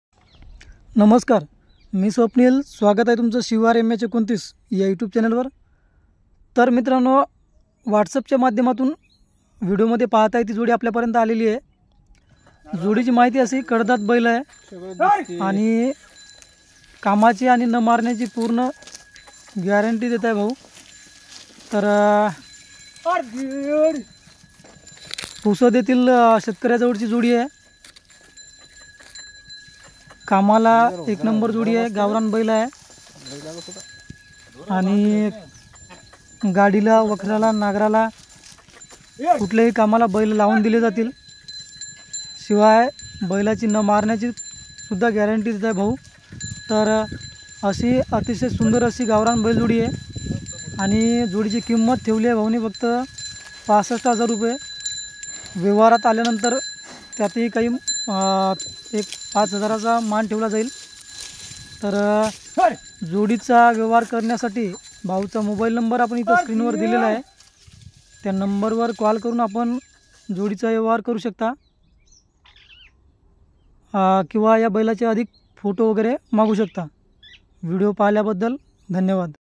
पुसद येथून सुंदर गावरान बैलजोड विक्री आहे
नमस्कार शेतकरी मित्रांनो अतिशय सुंदर अशी गावरान बैल जोडी आपल्यासाठी पुसद तालुक्यातून विक्रीसाठी उपलब्ध झालेली आहे जर कोणालाही जोडी घ्यायची असेल तर शेती कामासाठी एक नंबर बैल आहे जर घ्यायचे असेल तर भाऊचा मोबाईल नंबर आपण स्क्रीनवर ठेवलेला आहे त्या नंबर वर कॉल करून आपण या जोडीचा व्यवहार करू शकता अतिशय सुंदर गावरान शांत बैल आहे कामासाठी एक नंबर पहिला आहे तर बघा कुणाला आवडली तर घ्या